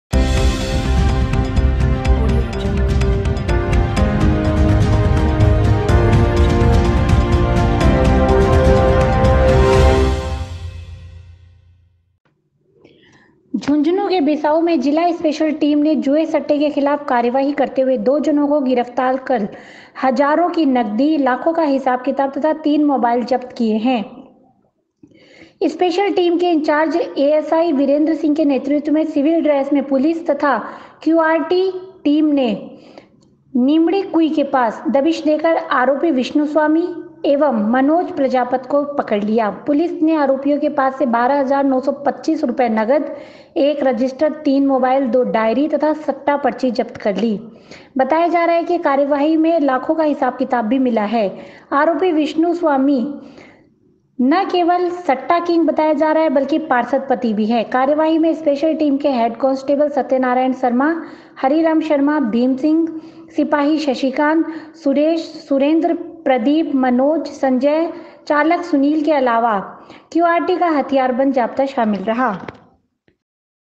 JHUNJHUNU I अंकों की खाईवाली करते हुए पार्षद पति गिरफ्तार
झुंझुनूं के बिसाउ में जिला स्पेशल टीम ने जुए सट्टे के खिलाफ कार्रवाई करते हुए दो जनों को गिरफ्तार कर हजारों की नगदी, लाखों का हिसाब किताब तथा तीन मोबाइल जप्त किए हैं। स्पेशल टीम के इंचार्ज एएसआई वीरेन्द्र सिंह के नेतृत्व में सिविल ड्रेस में पुलिस तथा क्यूआरटी टीम ने नीमड़ी कुई के पास दबिश देकर आरोपी विष्णु स्वामी एवं मनोज प्रजापत को पकड़ लिया। पुलिस ने आरोपियों के पास से 12 हजार 925 रुपए नगद, एक रजिस्टर, तीन मोबाइल, दो डायरी तथा सट्टा पर्ची जप्त कर ली। बताया जा रहा है कि कार्रवाई में लाखों का हिसाब किताब भी मिला है। गिरफ्तार आरोपी विष्णु स्वामी ना केवली सट्टा किंग बताया जा रहा है। बल्कि पार्षद पति भी है। कार्रवाई में स्पेशल टीम के हैड कांस्टेबल सत्यनारायण शर्मा, हरिराम शर्मा, भीम सिंह, सिपाही शशिकांत, सुरेश, सुरेन्द्र, प्रदीप, मनोज, संजय, चालक सुनील के अलावा क्यूआरटी का हथियारबंद जाप्ता शामिल रहा।